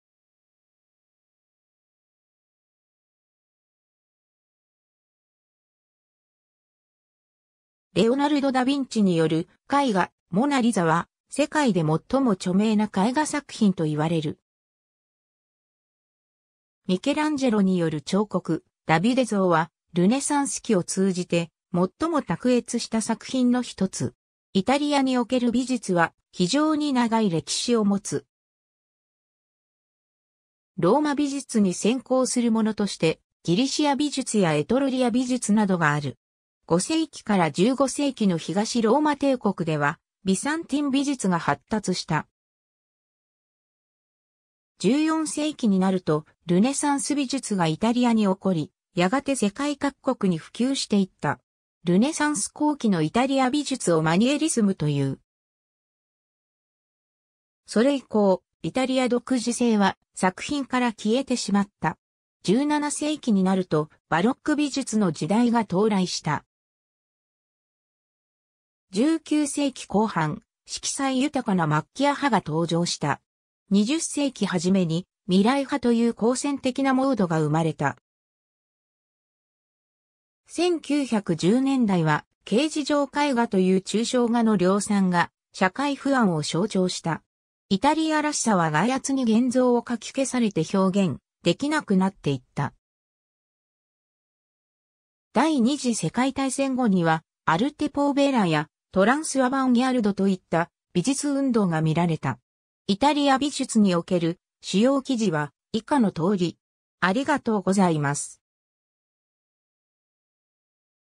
イタリア美術
レオナルド・ダヴィンチによる絵画「モナ・リザ」は、世界で最も著名な絵画作品といわれる。
 ミケランジェロによる彫刻「ダビデ像」は、ルネサンス期を通じて最も卓越した作品の1つ。
 イタリアにおける美術（英: Italian art）は、非常に長い歴史を持つ。
ローマ美術に先行するものとして、ギリシア美術やエトルリア美術などがある。
 5世紀から15世紀の東ローマ帝国ではビザンティン美術が発達した。
14世紀になるとルネサンス美術がイタリアに興り、やがて世界各国に普及していった。
ルネサンス後期のイタリア美術をマニエリスムという。
それ以降、イタリア独自性は作品から消えてしまった。
17世紀になると、バロック美術の時代が到来した。
 19世紀後半、色彩豊かなマッキア派が登場した。
20世紀はじめに未来派という好戦的なモードが生まれた。
1910年代は形而上絵画という抽象画の量産が社会不安を象徴した。
イタリアらしさは外圧に原像をかき消されて表現できなくなっていった。
 第二次世界大戦後には、アルテ・ポーヴェラやトランスアバンギャルドといった美術運動がみられた。
 イタリア美術における主要記事は以下の通り。